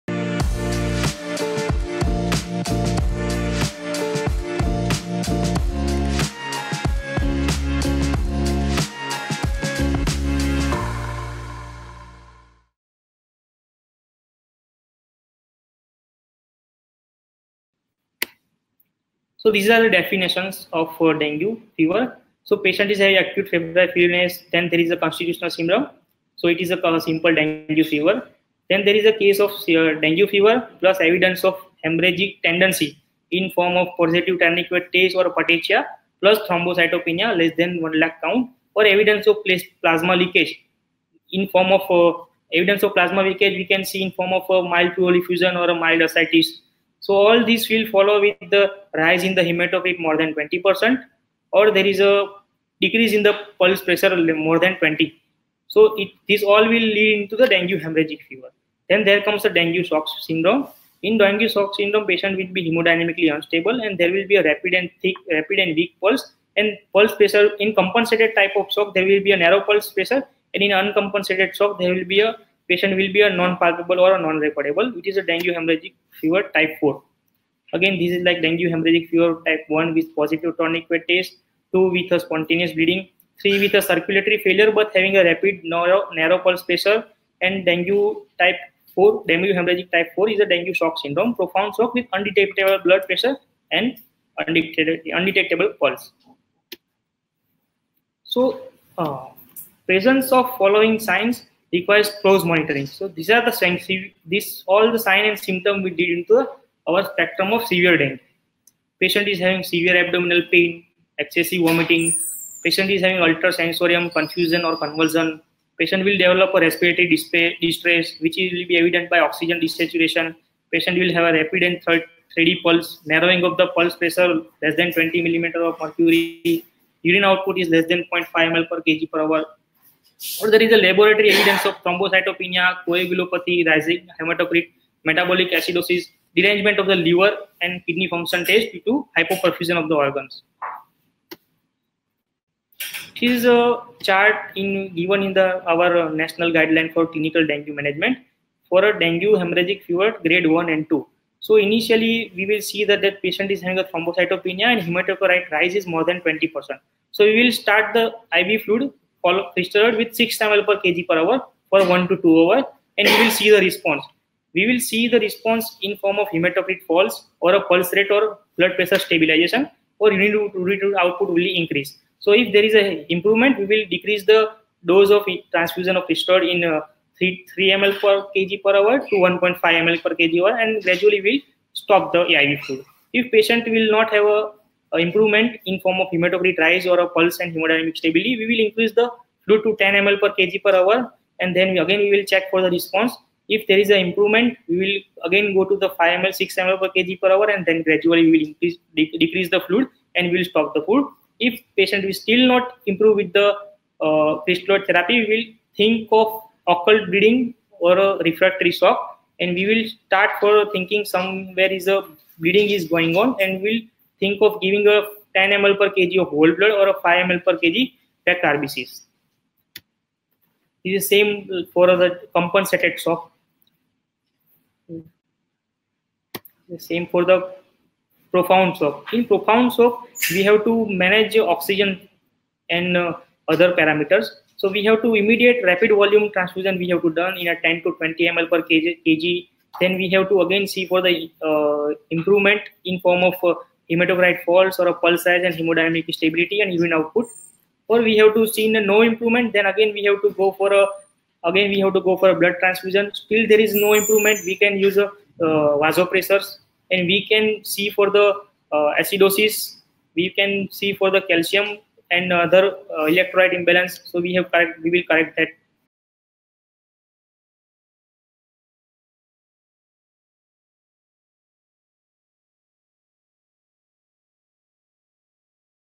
Clinical Features and Management of Dengue | Dr. Manoj K. Singh | Medvarsity
The clinical presentations of Dengue Fever (DF) go from asymptomatic to severe conditions that may even lead to death if not effectively managed. The symptomatic cases may be broadly categorized as undifferentiated febrile illness, dengue fever, unusual dengue fever, dengue shock syndrome, dengue hemorrhagic fever or expanded dengue syndrome.

To learn more about the symptoms and management of diverse dengue cases, tune in with Dr. Manoj K. Singh MD, DTCD, DNB (Chest), FNB (Critical Care), European Diploma of Critical Care (EDIC), FICCM, Apollo Hospitals, Ahmedabad.